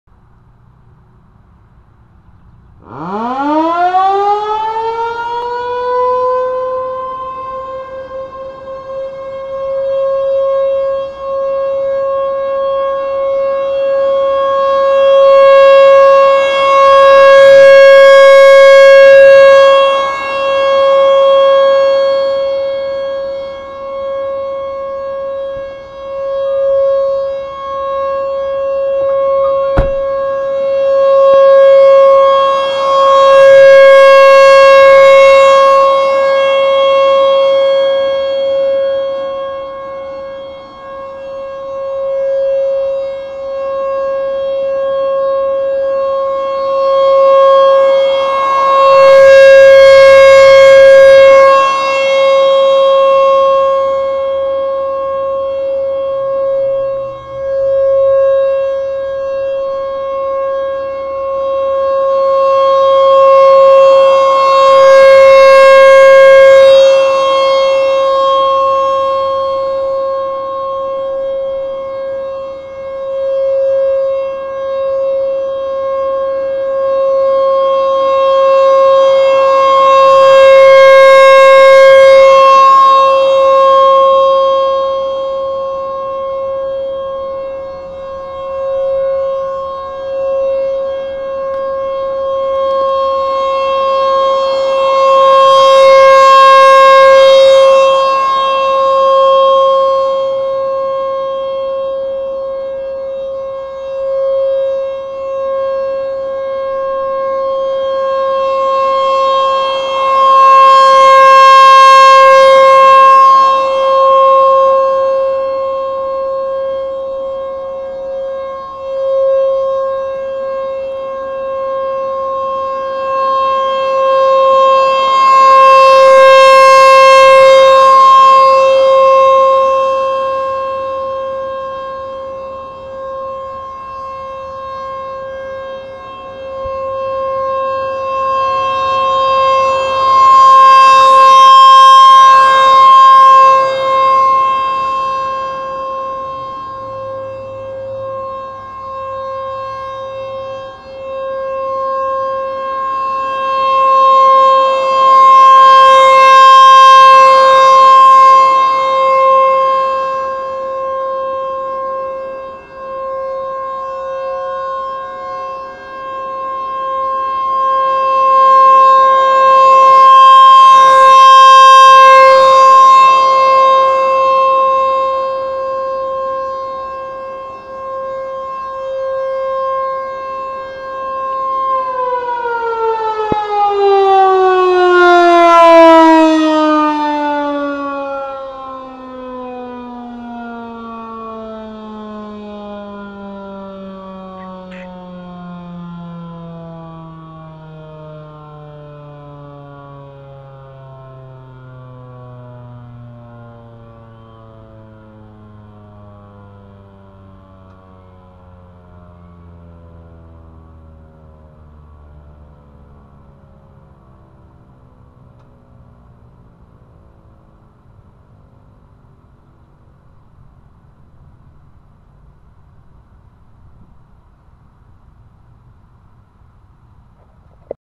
T-128 siren test, Omaha, NE.
Another T-128, with 4rpm rotation